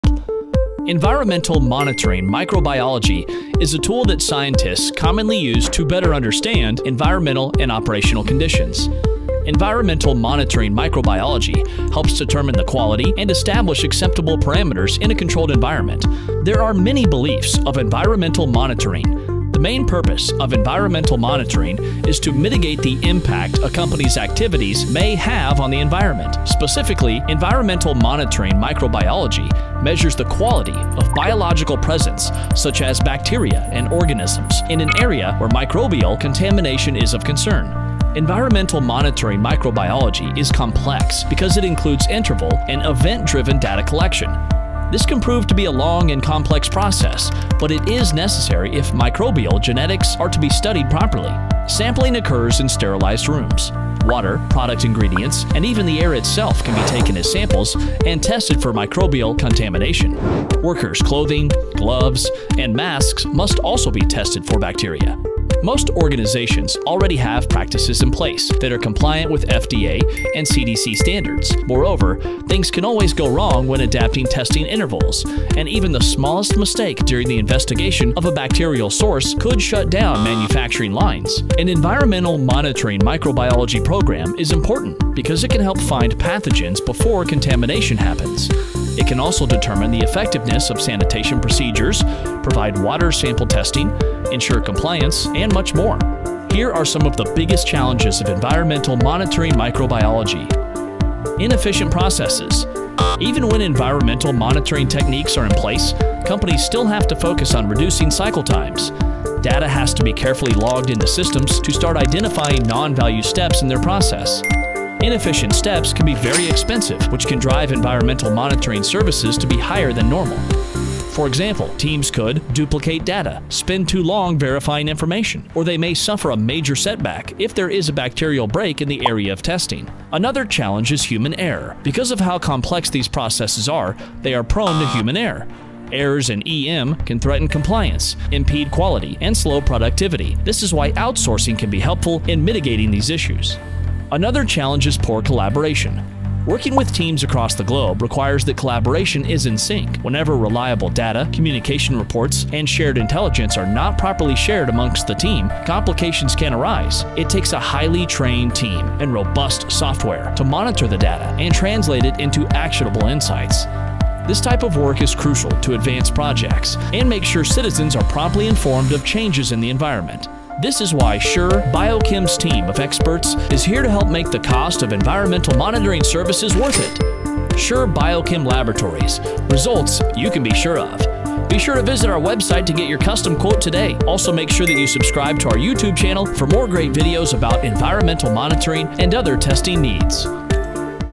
Biggest Environmental Monitoring Challenges to Consider
Environmental monitoring microbiology is extremely complex in nature.

It includes interval, event-driven data collection to study microbial genetics, it can be a long process.

Sampling occurs in sterilized rooms. Water, product ingredients, and even the air itself can be taken as samples and tested. Workers’ clothing, gloves, and masks must also be tested for bacteria.

Most organizations already have practices in place that are compliant with FDA and CDC standards, however, things can always go wrong when adapting testing intervals.

A mistake when investigating a bacterial source could shut down manufacturing lines!

With that said, this video breaks down the biggest challenges of environmental monitoring microbiology you must consider.